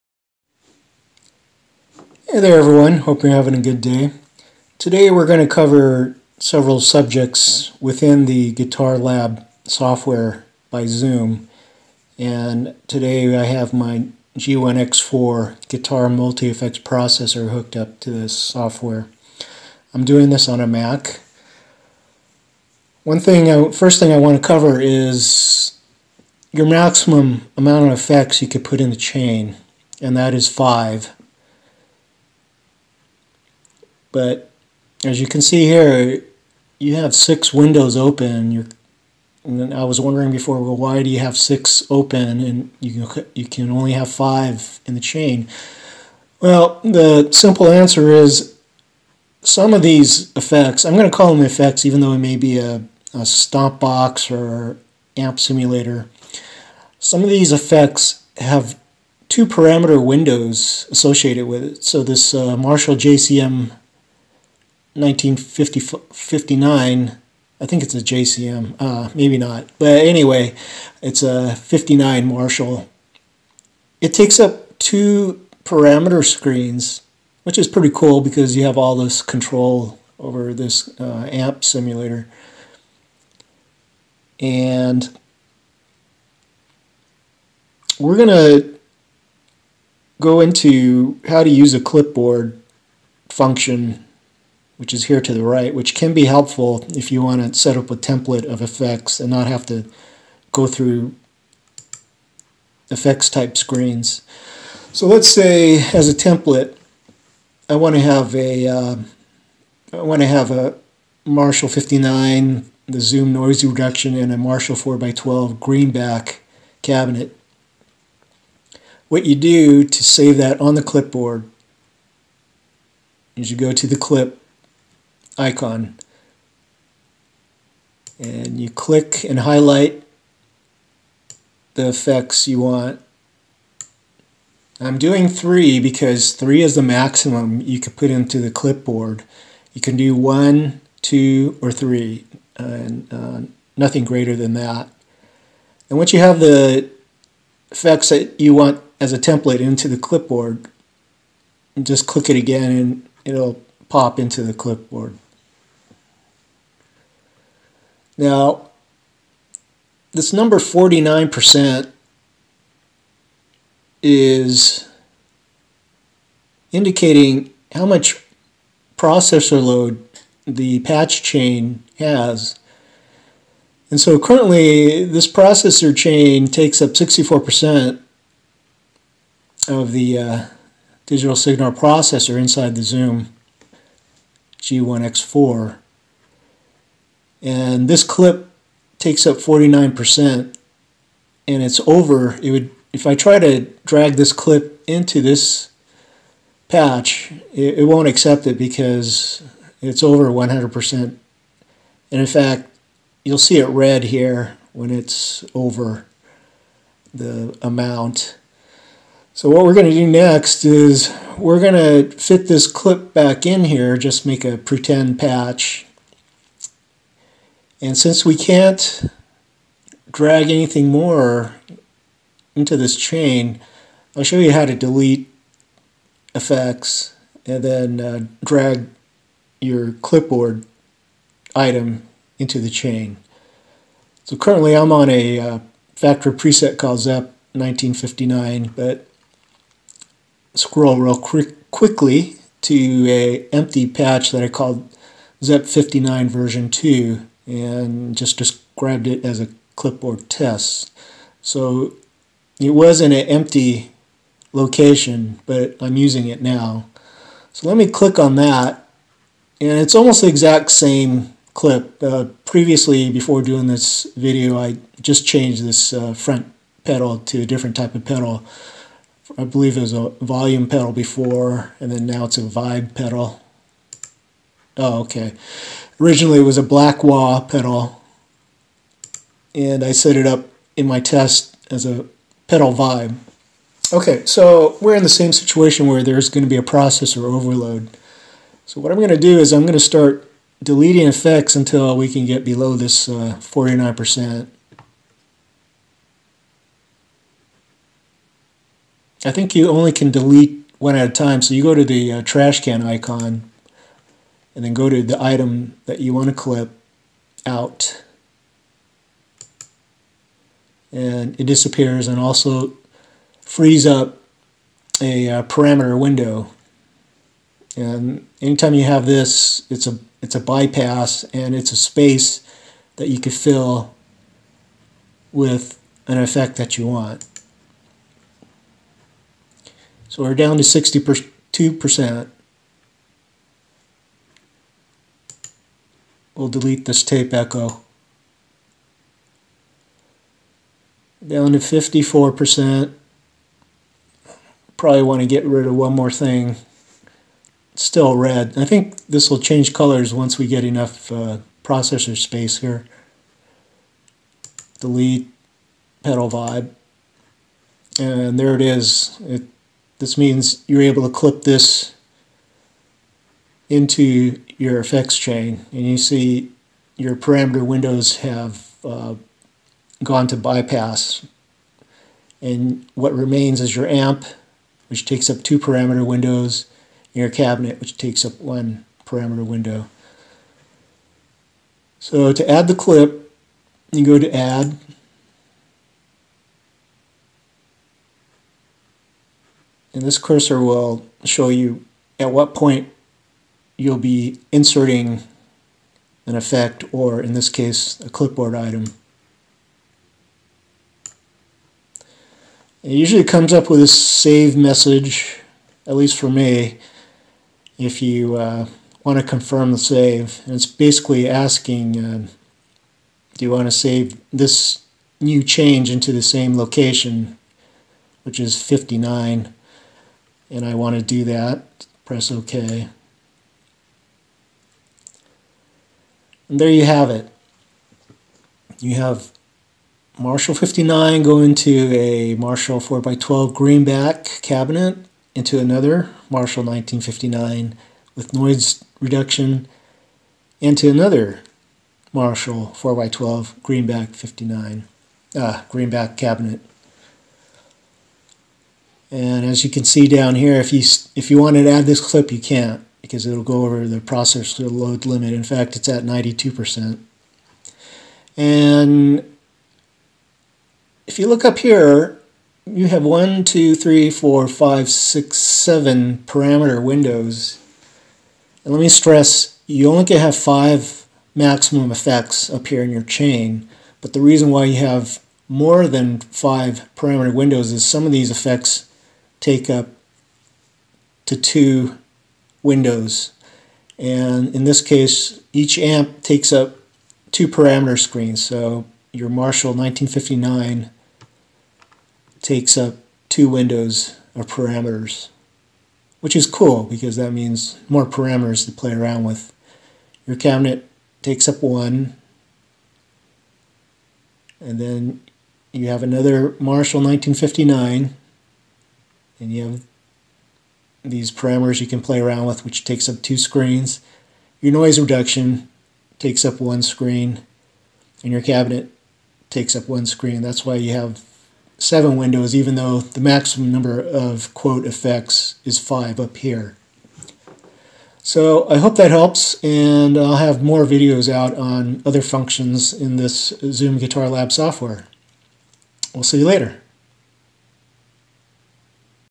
ZOOM G1X four MAX Processor Loading and Clipboard functions - GUITAR LAB 5.0 software - Tutorial
Video Series of easy tutorials for Zoom GUITAR LAB 5.0
How to use Clipboard and Processor max effects load

If you appreciate the info please donate to help grow my channel

Thanks
Sierra Signal Channel